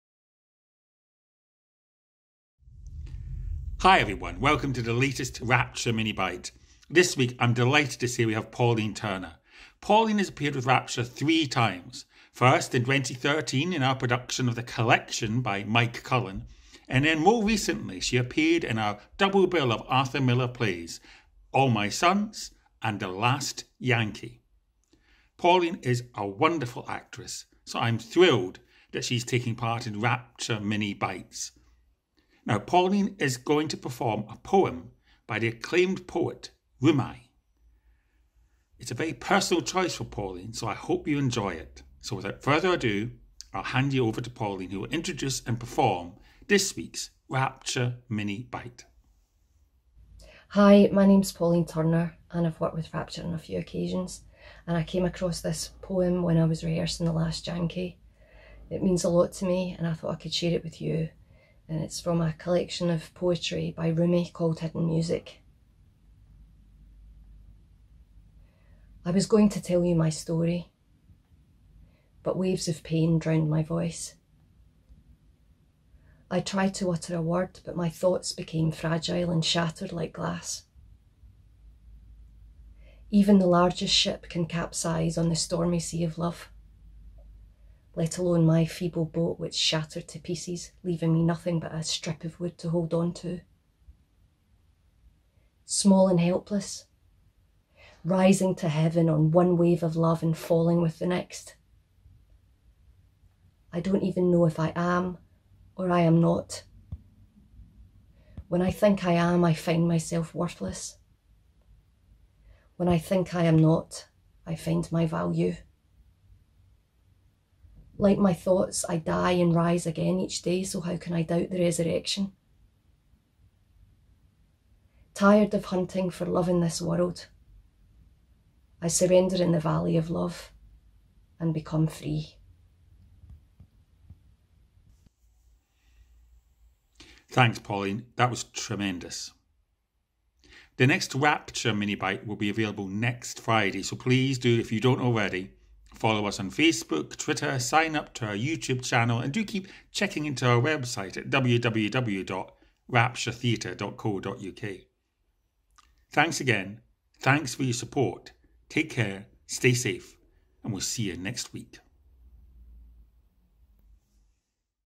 Pauline Turner - Rumi
The wonderful Pauline Turner recites from Rumi’s Hidden Music.